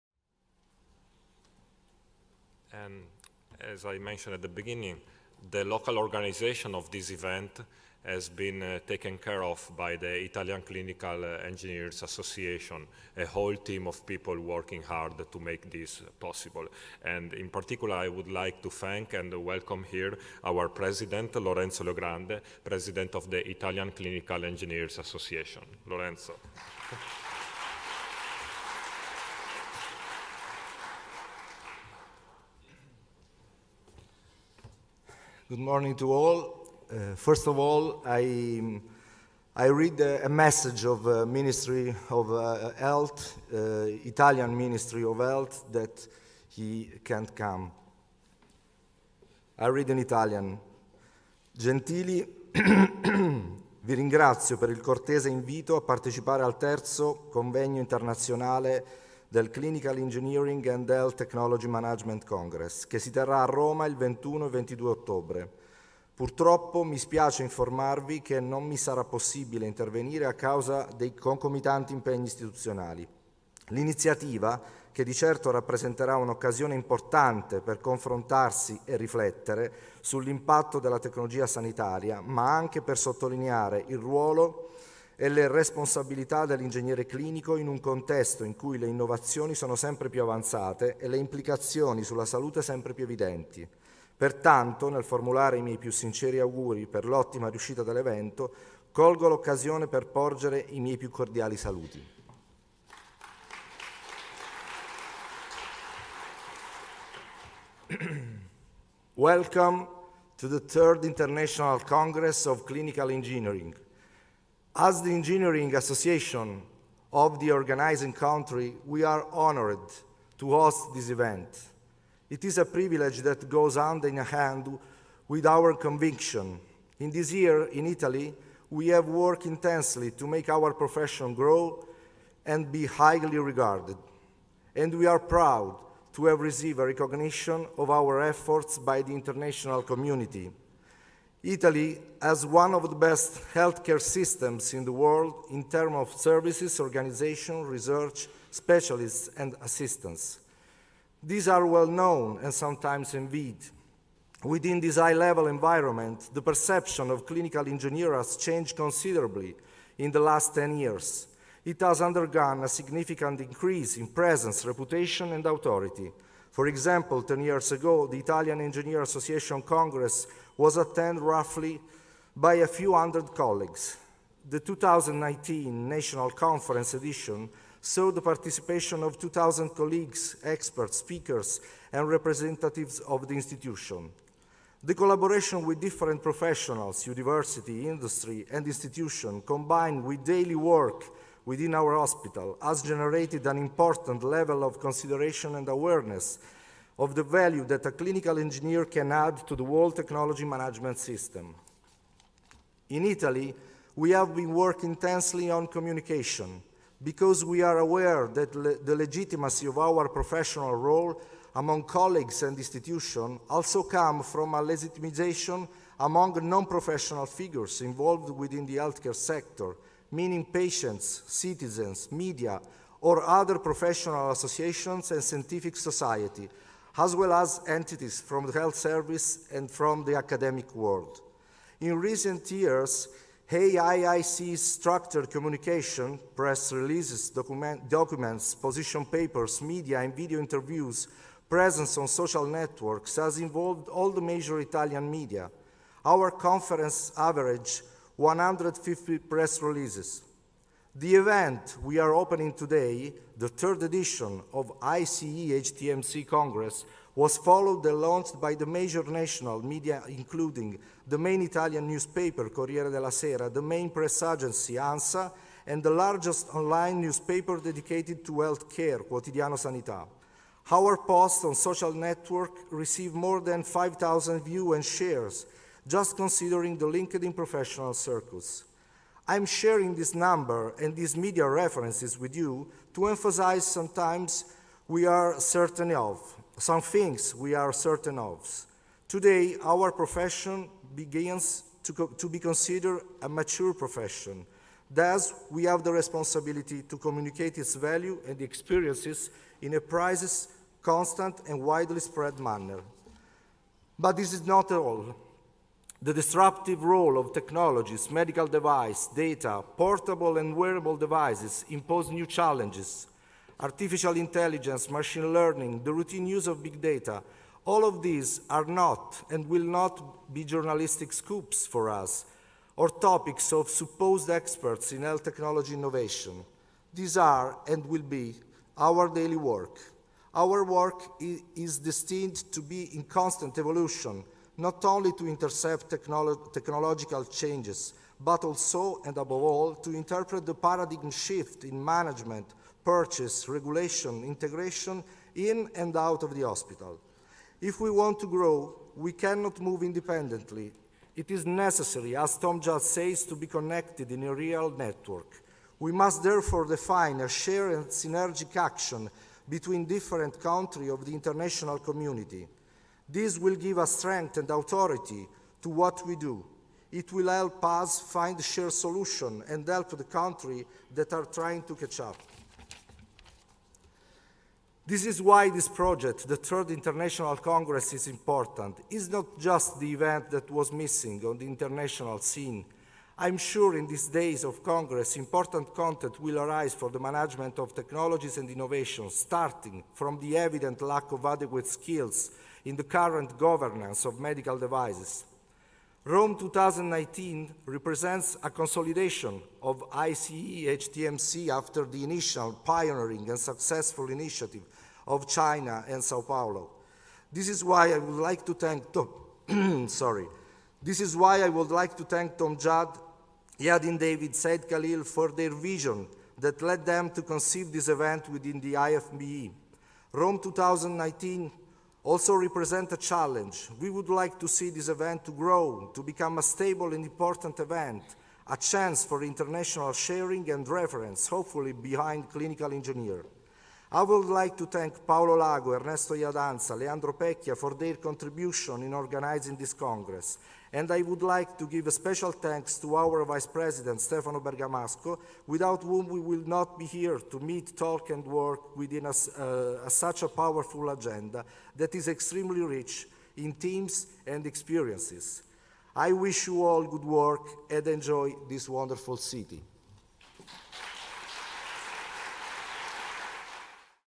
III ICEHTMC - Opening Ceremony Part 4 by Lorenzo Leogrande (October 21, 2019)
This is part 4/6 of the opening ceremony of the 3rd. International Clinical Engineering and Health Technology Management Congress held in Rome, Italy in October 2019.